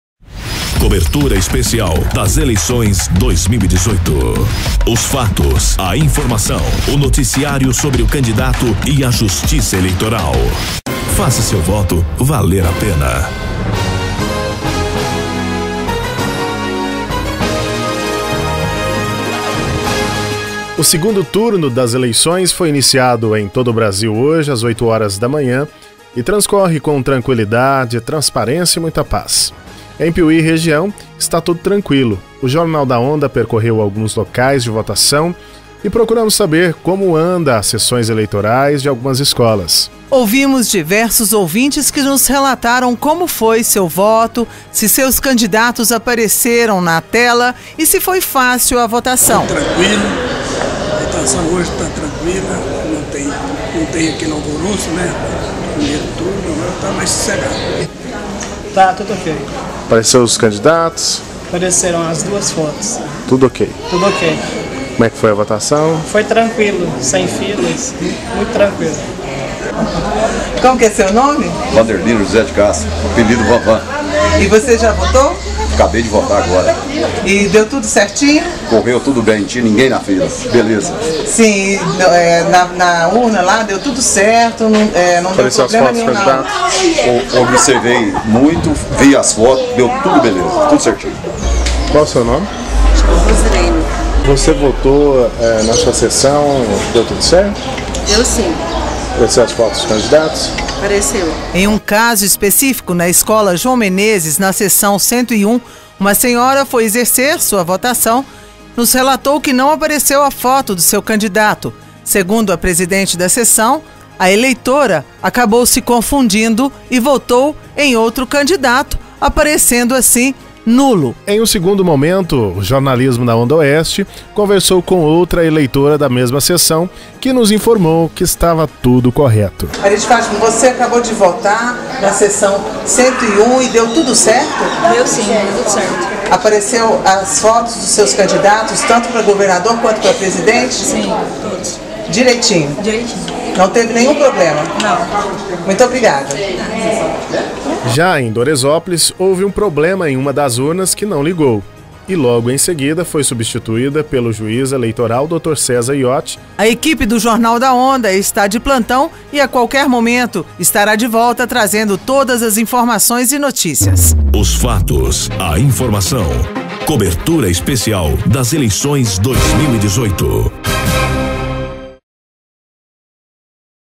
Eleições 2018 - Segundo-Turno primeiras informações de Minas
Segundo-Turno, Confira como está acontecendo a votação em todo pais, em minas, na região e em Piumhi.